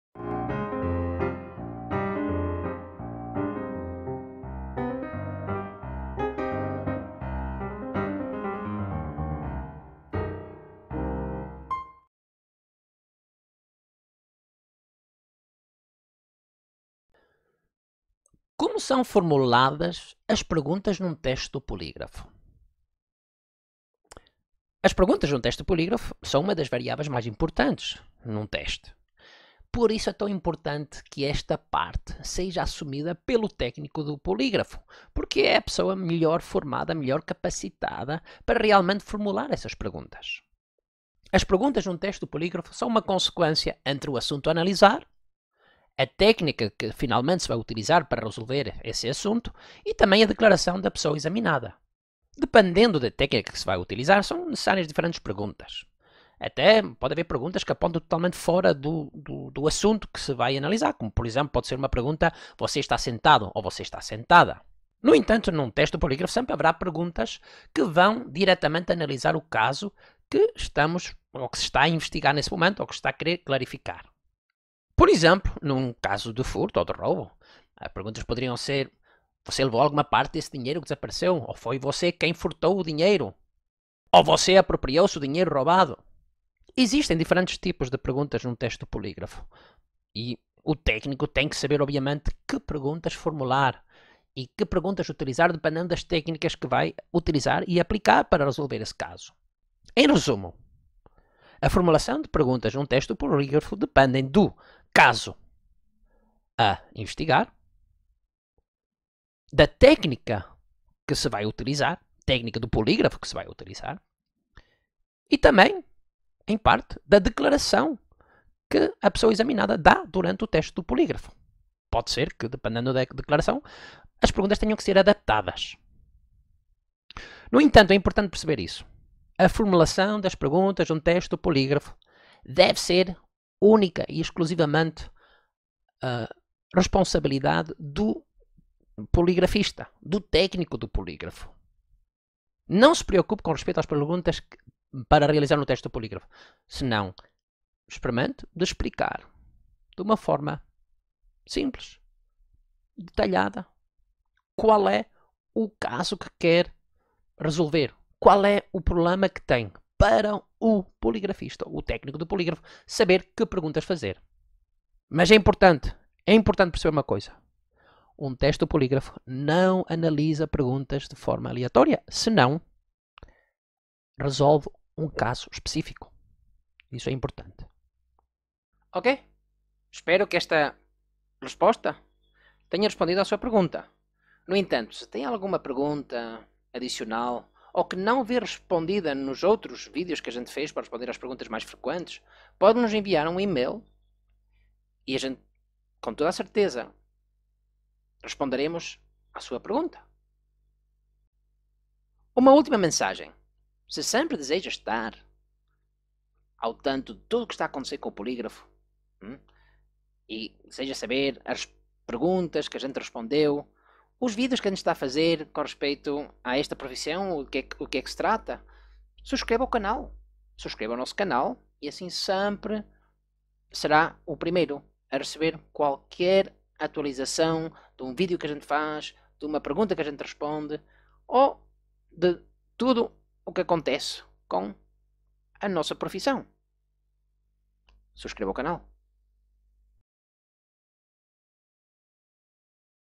Como formular perguntas num teste do polígrafo, detector de mentiras, máquina da verdade?
Como são formuladas as perguntas de um teste de polígrafo, detector de mentiras ou máquina da verdade?

As perguntas de um teste de polígrafo são uma das variáveis mais importantes quando se trata fazer um exame de polígrafo. É por isso que é tão importante que é seja o técnico do polígrafo quem os formula.

As perguntas são uma consequência do assunto a ser analisado e da técnica de polígrafo utilizada.

Dependendo da técnica, são necessários diferentes tipos de perguntas que podem até apontar fora do assunto investigado, como por exemplo: Você está sentado?

No entanto, sempre há perguntas que serão feitas para considerar o assunto a ser analisado.

Por exemplo, em um teste de furto / roubo pode ser algo como:

- Você levou alguma parte do dinheiro?
- Foi você quem furtou o dinheiro?
- Você apropriou-se do dinheiro roubado?

Existem diferentes tipos de perguntas num teste de polígrafo e o técnico deve saber como formular cada uma para o tipo de assunto que analisa.

Portanto, a formulação das perguntas depende:

1. Técnica utilizada
2. Assunto investigado
3. Declaração da pessoa examinada

A formulação de perguntas deve ser única e exclusivamente a responsabilidade do técnico. O importante para a parte que contrata o exame é definir o assunto do exame e saber explicar o caso, o técnico saberá quais perguntas fazer.

Video realizado por Amável Sanches, psicofisiologo forense, perito do polígrafo, membro da Associação Americana de Polígrafo, criminalista forense com pos-graduação pela Universidade Autónoma de Barcelona.

Obrigado,
B.E.A.R. FORENSICS